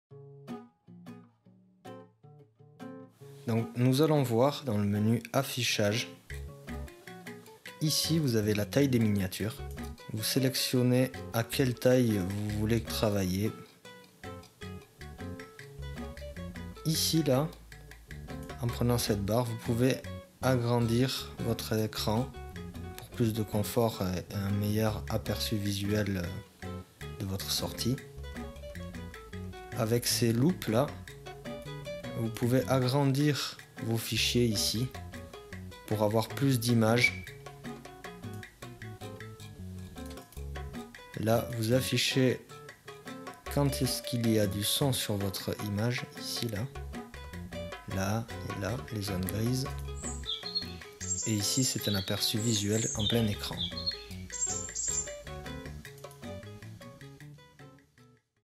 movie maker tutoriel: onglet affichage
movie maker tuto: découverte de l'onglet affichage,  personnaliser l'affichage de votre espace de travail